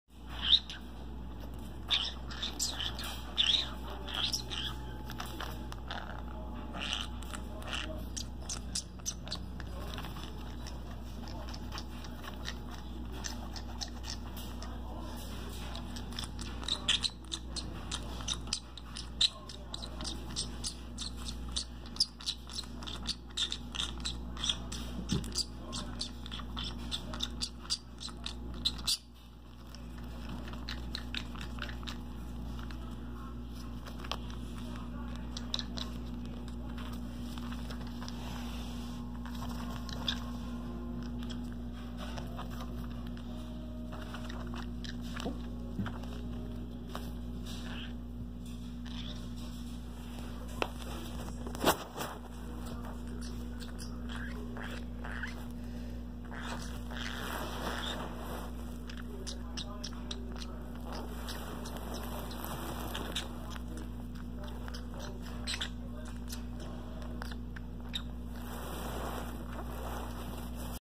Corky is freaky
Corky has an obsession with camouflage clothing. He appears to be claiming it as his own? I wonder why exactly he is attracted to this type of clothing. The color perhaps? He has his reasons, but I find it quite odd as he can get pretty aggressive over this clothing. Normally we put this clothing away (out of his sight) but this jacket was forgotten about today. Corky spotted it and well you know the rest.